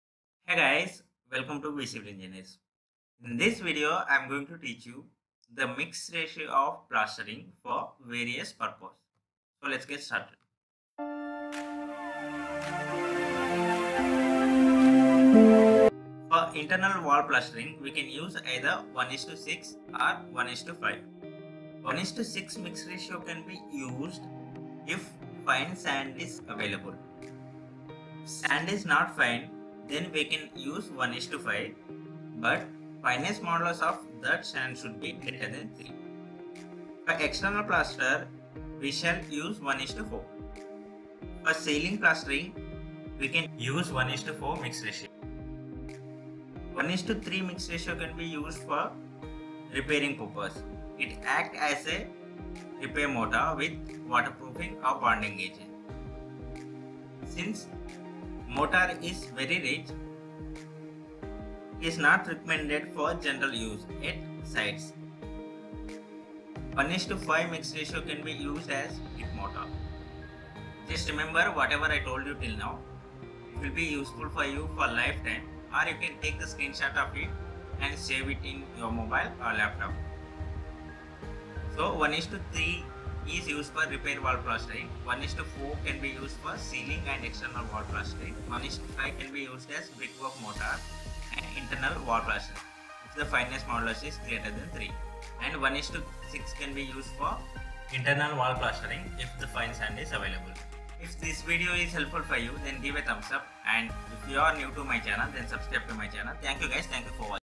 Mix ratio of plastering for various purpose
In this video, I have explained: "Mix ratio of plastering/mortar for various purpose, Where 1:3, 1:4, 1:5 & 1:6 mix ratio can be used?".

From this video, You can learn the following points:
1.Mix ratio for internal wall plastering
2. Mix ratio for external wall plastering
3. Mix ratio for ceiling plastering

So, watch it & learn it.
Happy learning :-)